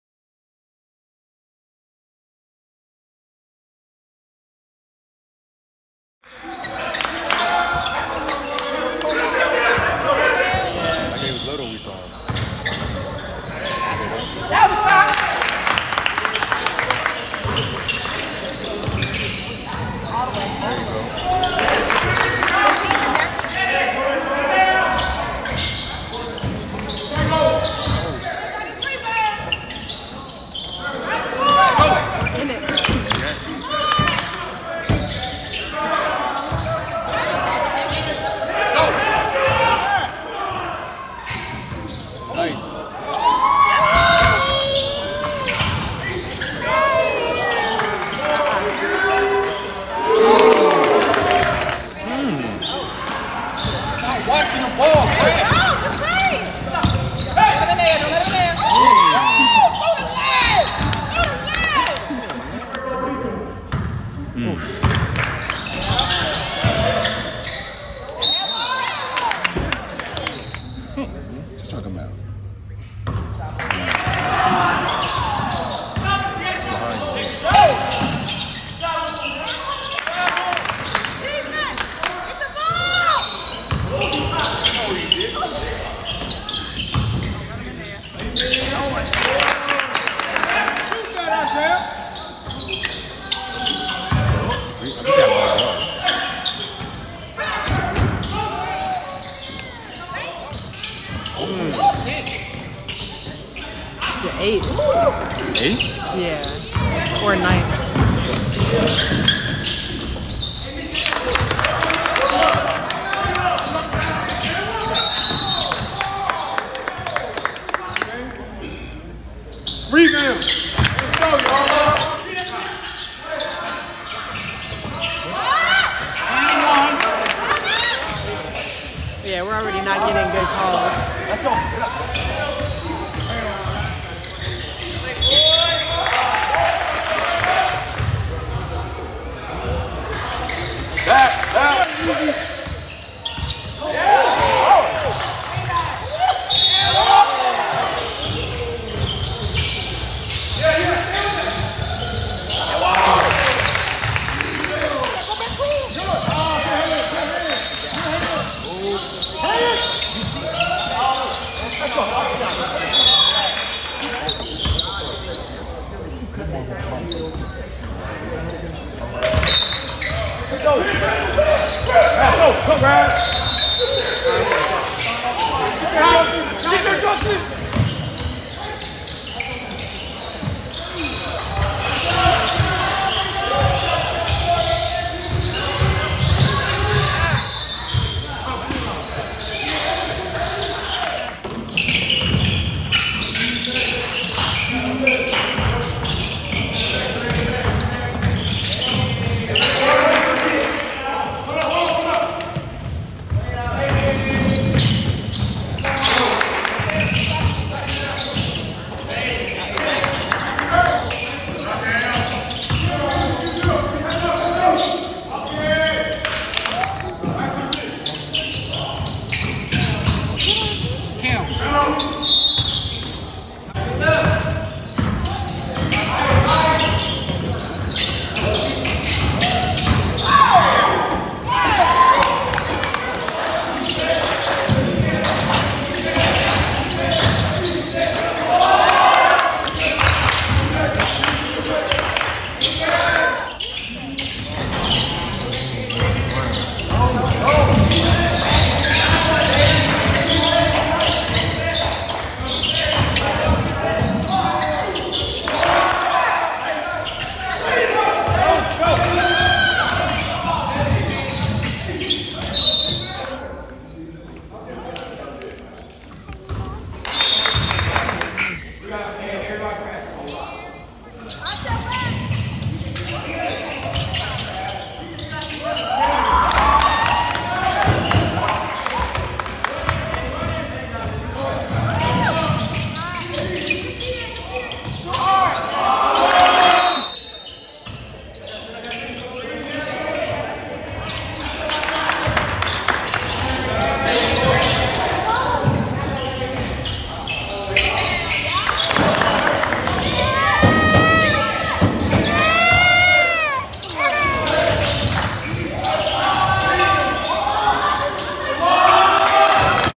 Timothy Allen Crank Highlights - Jr. Peach State Showcase - Augusta, GA - July 2024
Timothy Allen Crank
X: @timmyallencrank

6’0” Point Guard
Class of 2027
Saint John Paul the Great High School
Dumfries, Virginia
4.0 GPA

Team Sauce